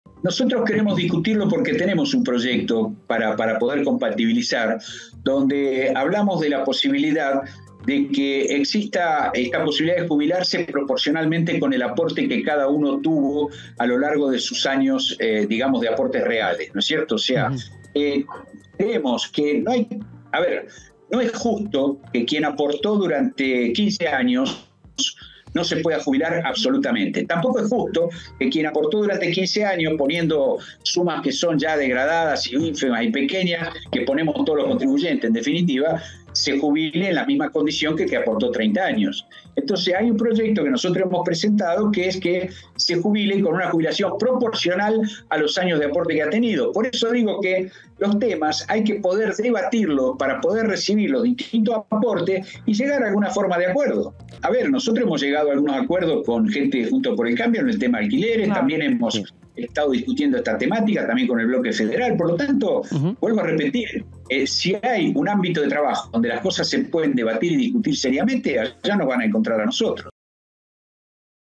Proponen avanzar con Moratoria Previsional, Hidrógeno Verde, Blanqueo, Monotributo Tech
La Cámara baja ingresa en el período extraordinario y legisladores del oficialismo y de la oposición anticipan cuáles son las iniciativas que les gustaría debatir, tanto en comisiones como en el recinto.

Desde esta semana, la Cámara de Diputados de la Nación, que conduce Cecilia Moreau, inicia el período de sesiones extraordinarias establecido por el Poder Ejecutivo Nacional. Por tal motivo, legisladores oficialistas y opositores analizan cuáles son los proyectos del temario, también propuesto por el Gobierno Nacional, que les gustaría tratar.

Sobre la iniciativa de Moratoria Previsional, en diálogo con el canal Diputados TV, Luis Di Giácomo (Juntos Somos Rio Negro) sostuvo: "Nosotros queremos discutirlo porque tenemos un proyecto para poder compatibilizar, en el que hablamos de que exista la posibilidad de jubilarse proporcionalmente con el aporte que cada uno tuvo a lo largo de sus años de aportes reales".

"No es justo que quien aportó durante 15 años no se pueda jubilar, como tampoco lo es que la persona que haya aportado ese tiempo se jubile en las mismas condiciones que quien lo hizo por 30 años. Entonces, nosotros lo que proponemos es que se jubile con una jubilación proporcional a los años de aporte que ha tenido", finalizó.